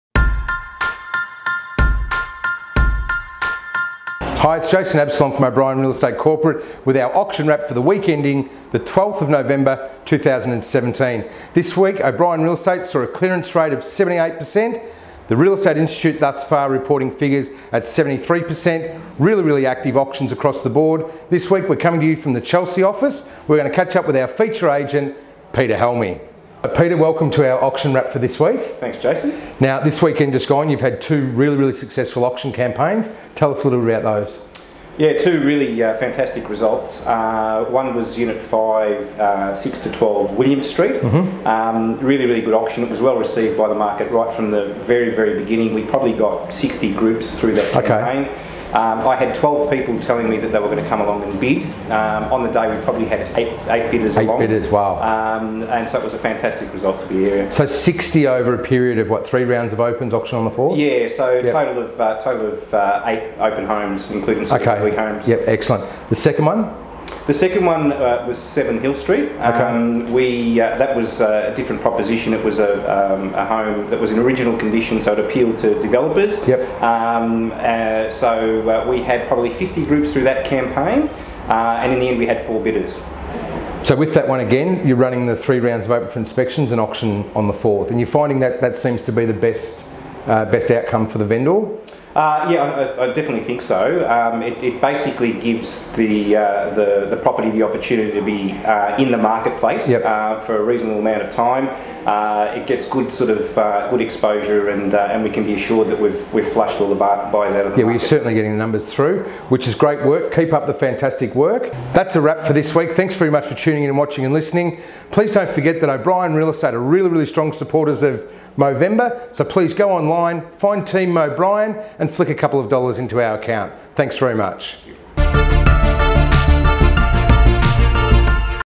Auction Wrap - 11th & 12th November
This week OBrien Real Estate achieved a 78% clearance rate with a stand out auction in the Bayside region receiving 83 bids in 11 minutes, that’s a bid every 7.9 seconds, it certainly kept our auctioneer busy.